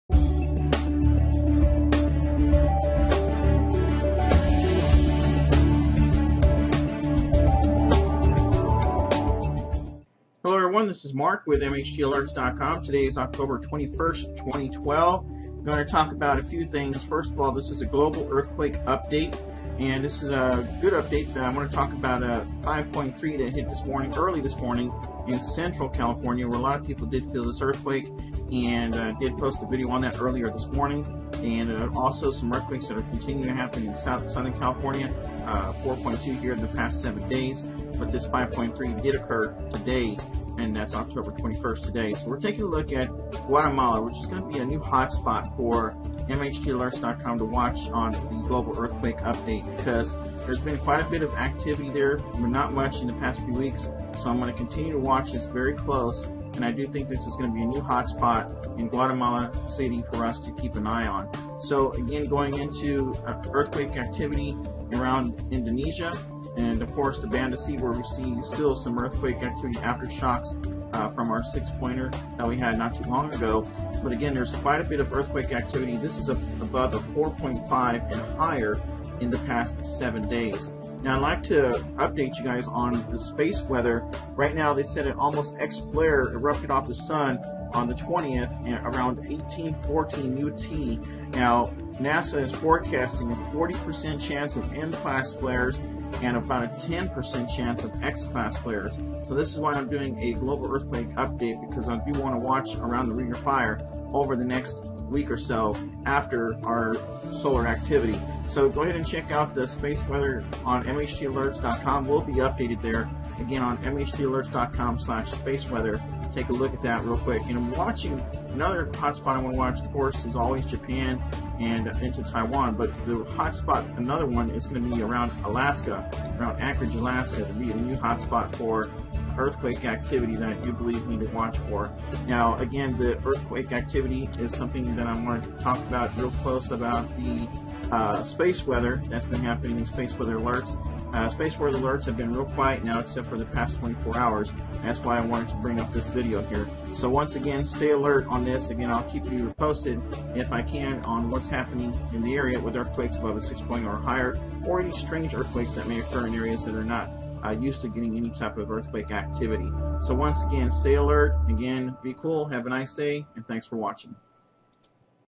Global Earthquake Update and Alert
Spaceweather is on the move again with a 10% chance of Xclass flares and a 40% chance of M class flares in the next few days.  Please stay alrert and keep track of our earthquake activity over the next few days.